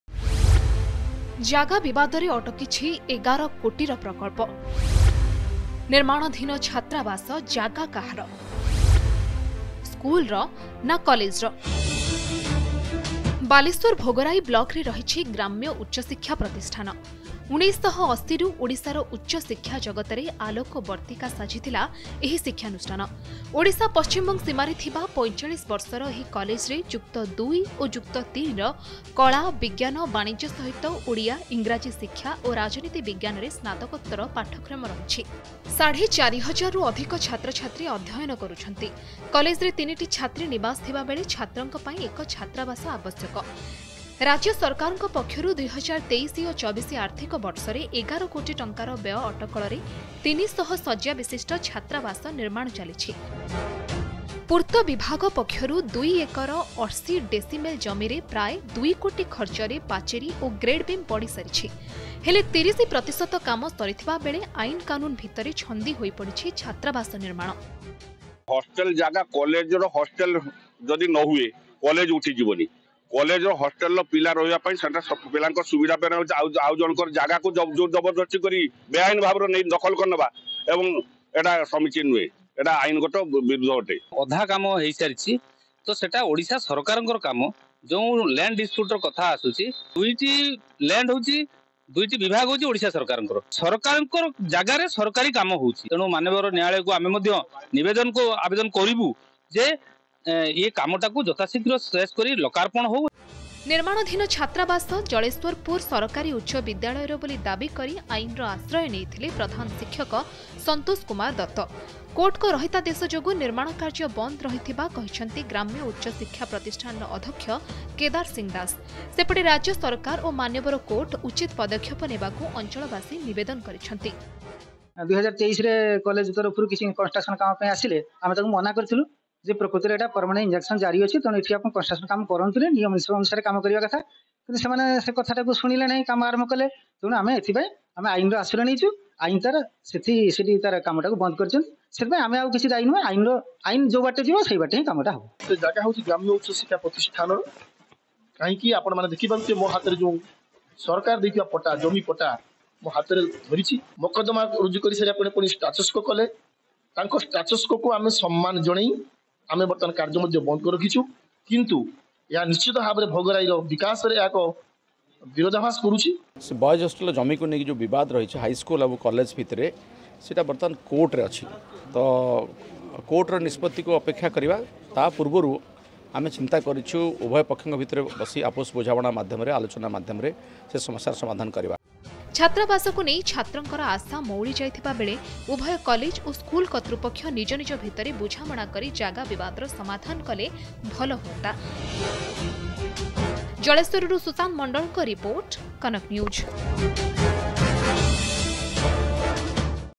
Rs 11 Lakh Project Stalled Due To Land Dispute In Balasore
A significant project worth Rs 11 lakh has come to a standstill in Balasore due to an ongoing land dispute.